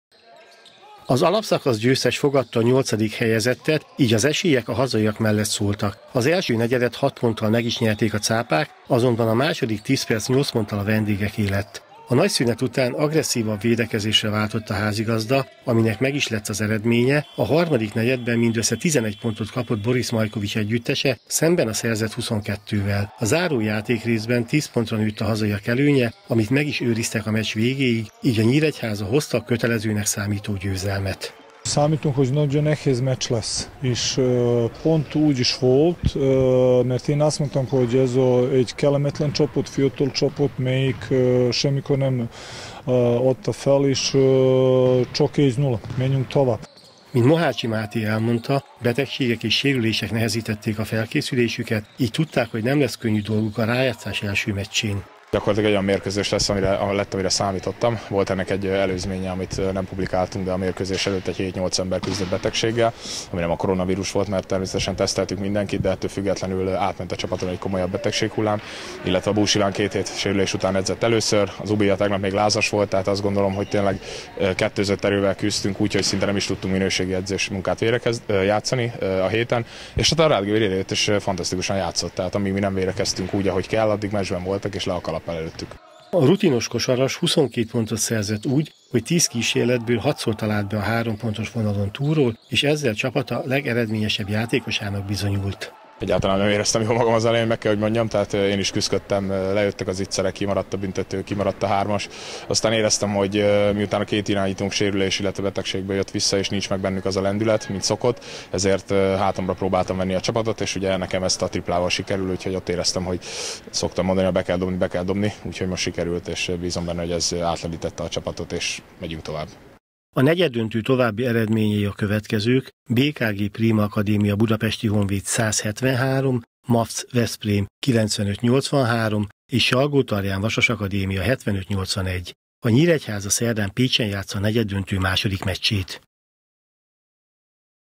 Előnyben a Cápák
Megnyerte a negyeddöntő első mérkőzését a Hübner-Nyíregyháza Blue Sharks. A Kék Cápák az NBI/B Piros csoportjának rájátszásában az NKA-Pécs csapatát verték 81-71-re. Az egyik fél harmadik sikeréig tartó párharc második meccsét szerdán rendezik Pécsen.